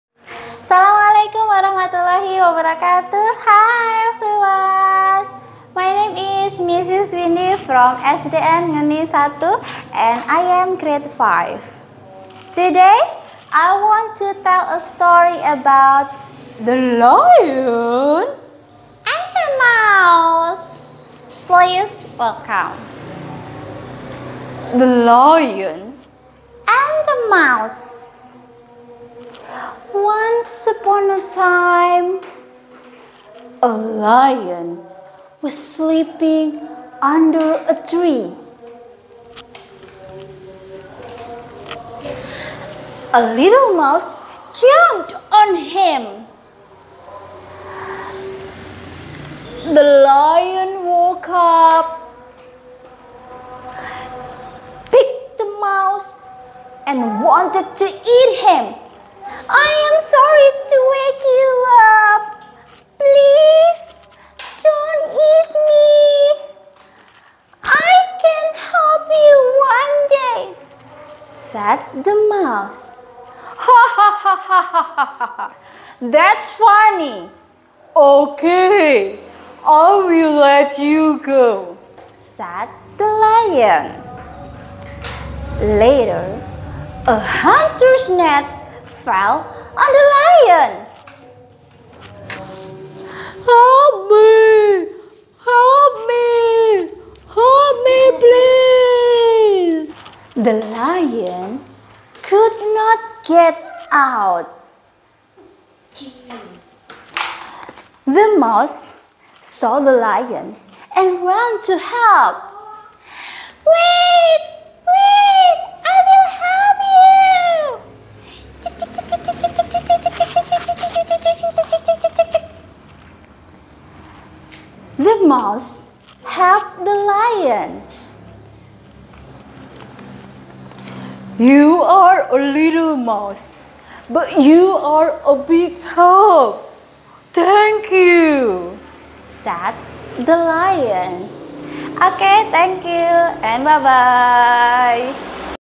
Story telling of The Lion and The Mouse
Latihan story telling untuk anak SD.